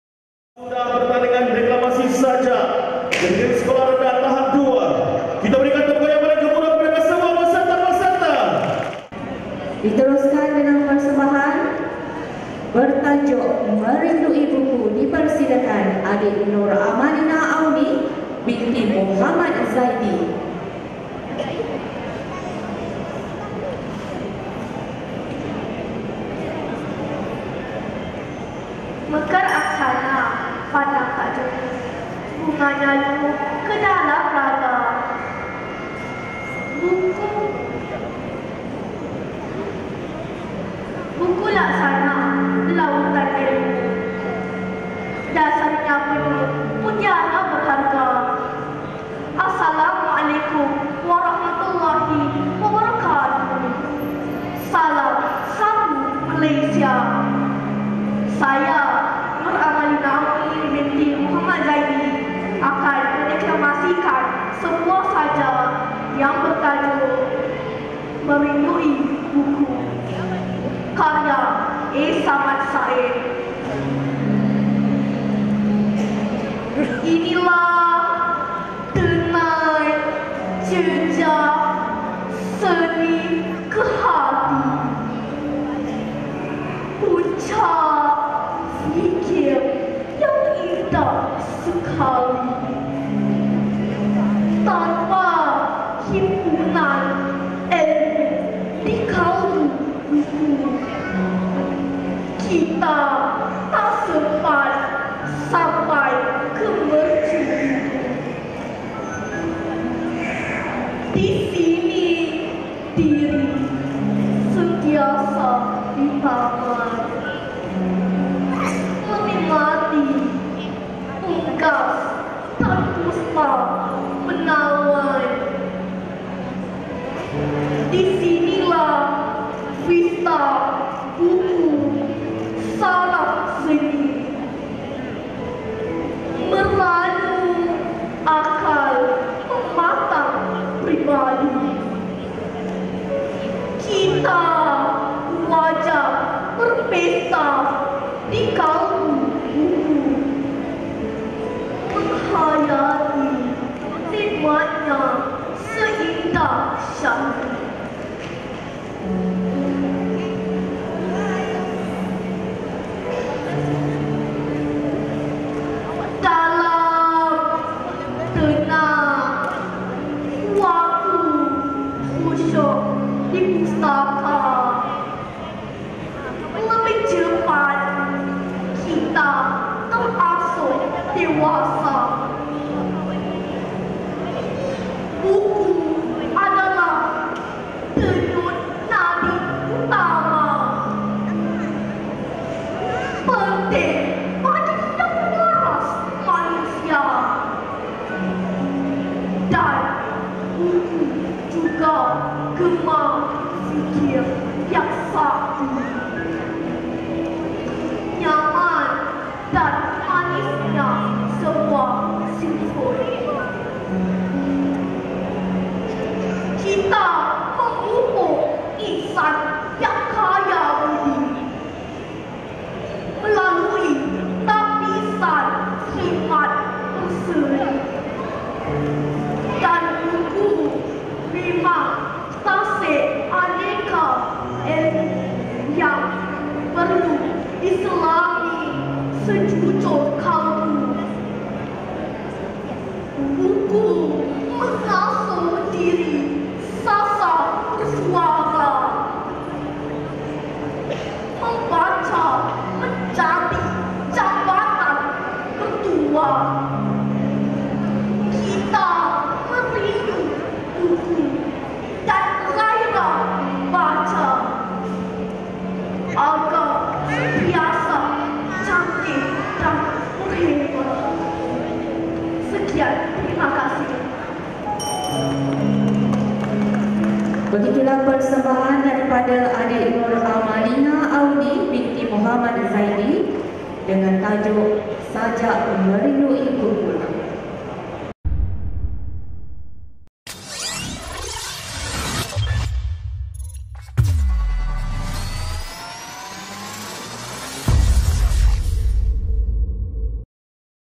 Pertandingan Deklamasi Sajak - Nuramalina Auni - SK Duyong
Pertandingan Deklamasi Sajak Sekolah Rendah Tahap 2 sempena Pesta Buku Melaka 2023
Nama : Nuramalina Auni binti Mohd Zaidy
Sekolah : SK Duyong
Tajuk : Merindui Buku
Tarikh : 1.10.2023
Tempat : The Shore Shopping Gallery